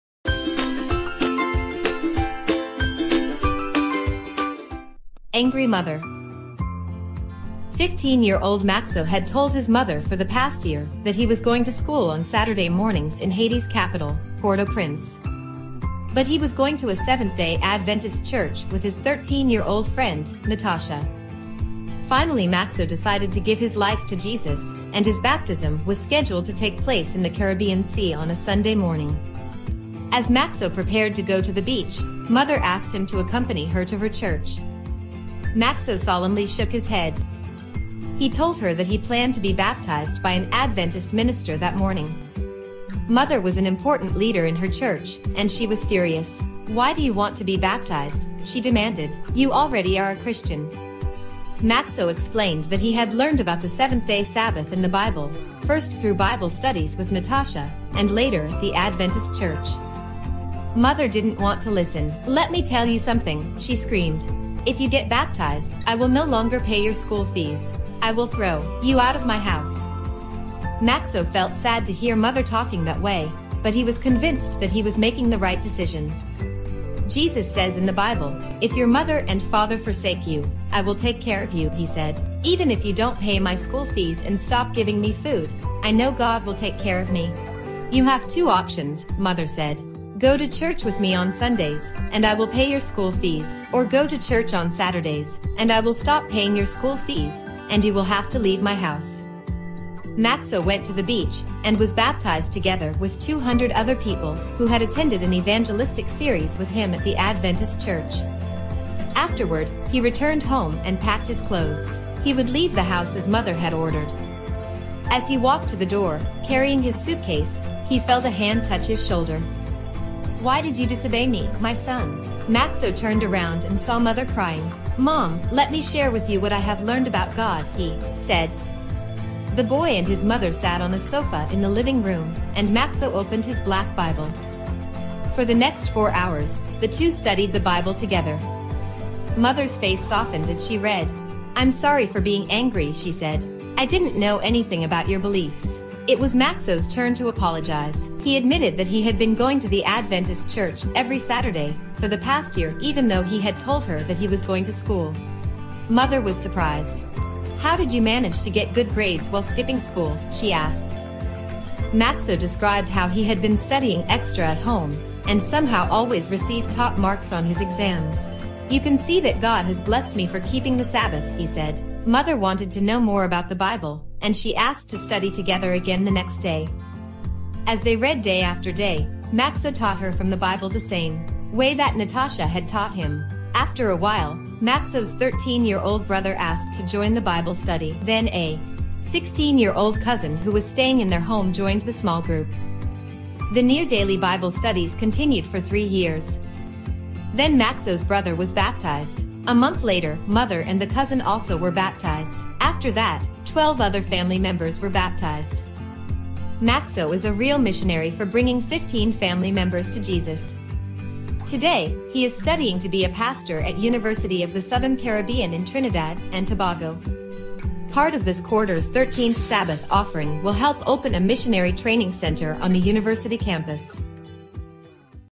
Adventist Mission Children's Stories April 10 Angry Mother
Seventh-day Adventist children Mission Story.

Thanks to the team at Renova 2.

Have fun!